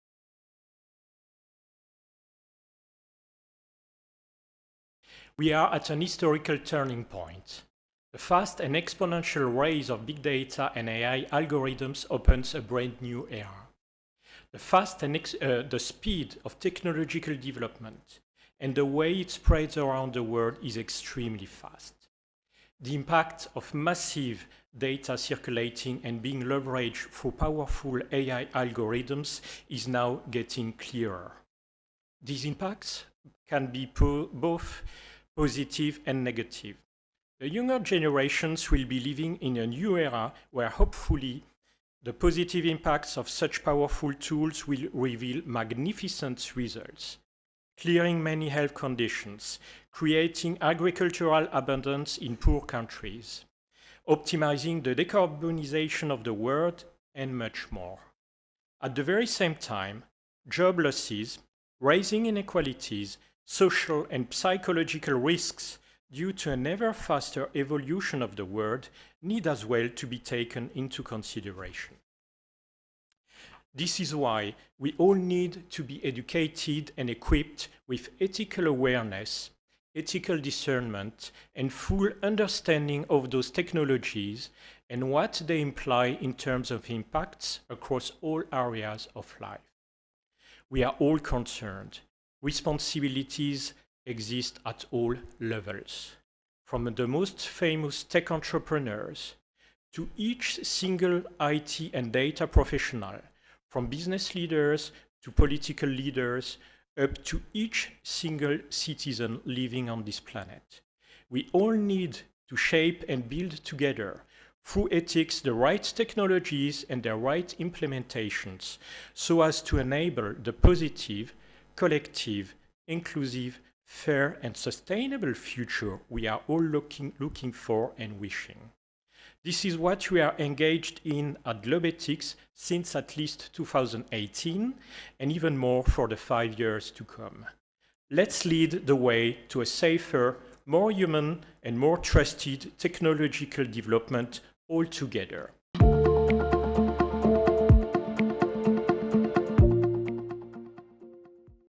Ethics of Digital and Emerging Technologies: Gilles Bach
Strategist/Consultant in the Big Data, AI, and IoT space and Globethics course instructor and cyber ethics expert Gilles Bach talks about the impact - both positive and negative - of AI and data developments and calls for all to be responsible for building ethical frameworks for digital and emerging technology.